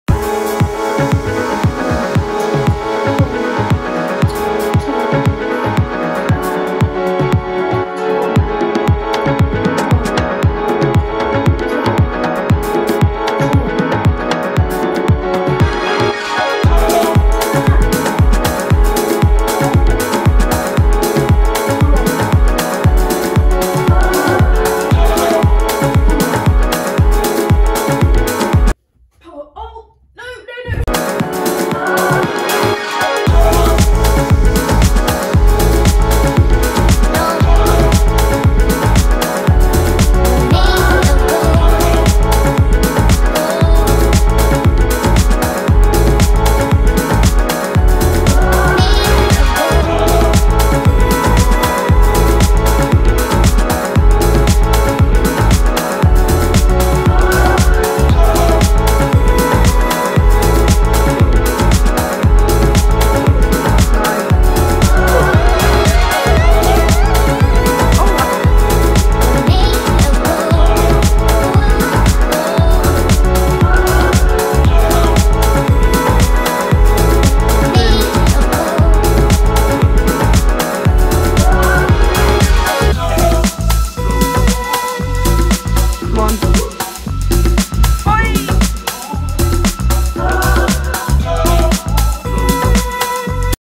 Tek's toilet roll challenge
Put those extra toilet rolls to use and test your high section kicking skills. Katy vs Sunny with a bonus attempt from Dr. Tom McCulloch, great effort Tom.🧻🦶🏻